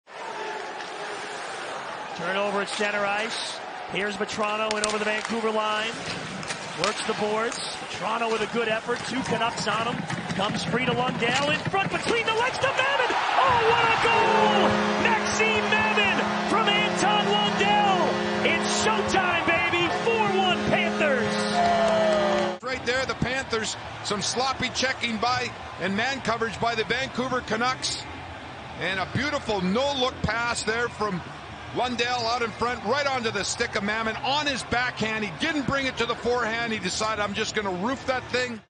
ФЛО-ВАН: у Мамина получился бросок с неудобной руки
Максим Мамин отправляет шайбу в девятку с острого угла с неудобной руки, забивая второй гол "Пэнтерз" за полминуты - 4:1